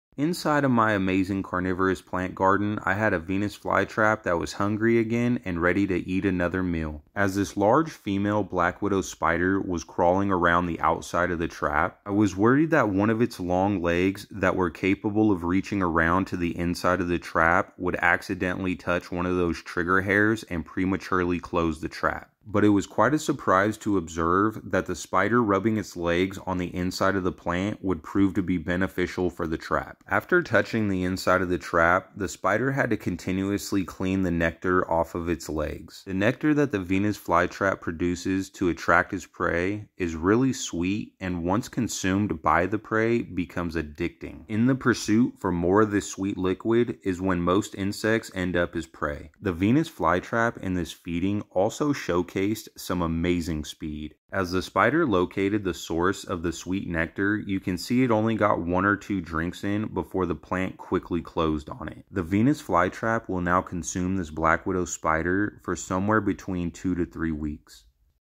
Can the Venus fly trap catch the black widow spider?
My Venus fly trap absolutely obliterated it's meal. It made super quick work of the black widow spider that came looking for more nectar. The speed the trap showed when closing on the spider was lightning quick, and surprising to see. I was especially happy with this capture as it was from a carnivorous plant that I have been growing under artificial light. That speed tome was a sign of great health the Venus fly trap. The spider will now be consumed for between 2 to 3 weeks. Really hoping with the health that the plant showed that it is closer to that 2 week mark.